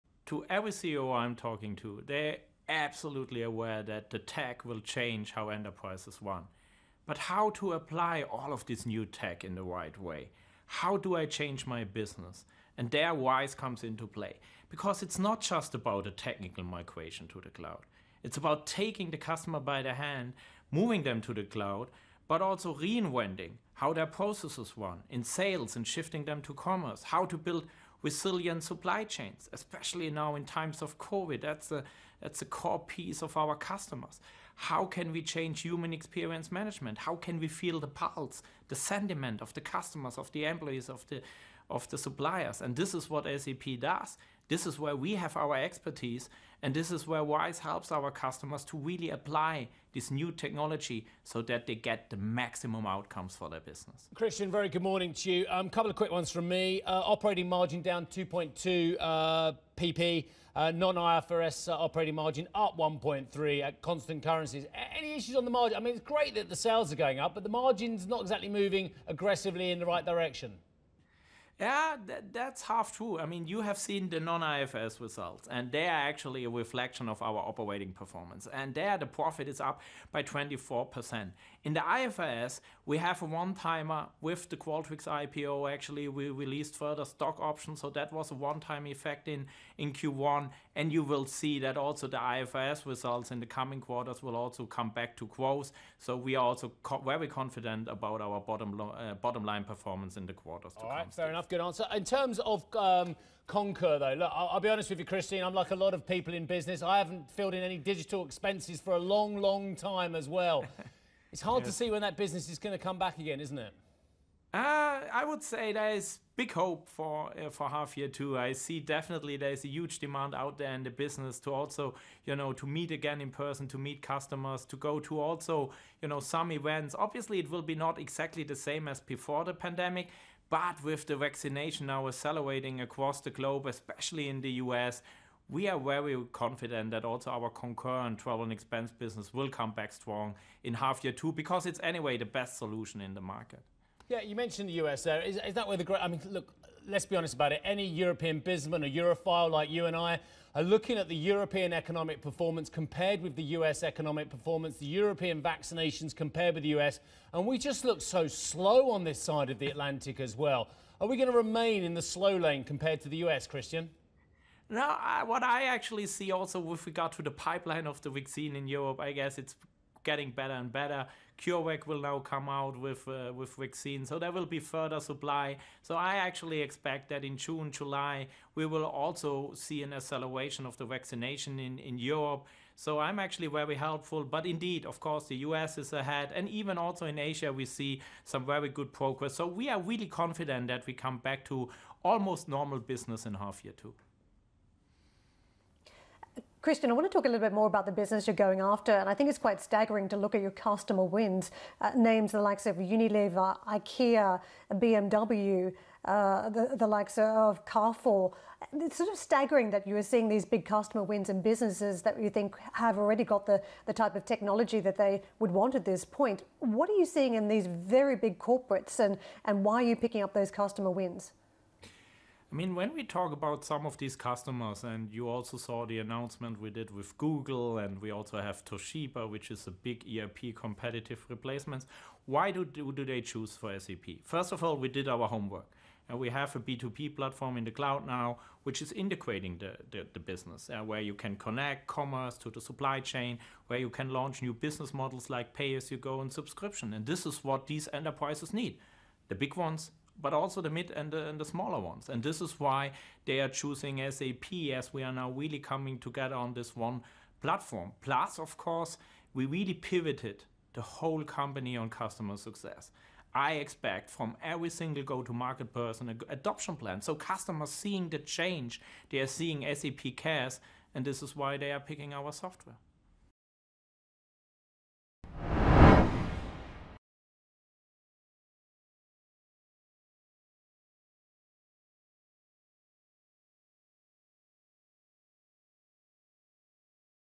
SAP CEO: We'll return to 'almost normal' in the second half of 2021
SAP CEO Christian Klein discusses the software company's first-quarter earnings, the global transition to the cloud and the outlook for the second half of 2021.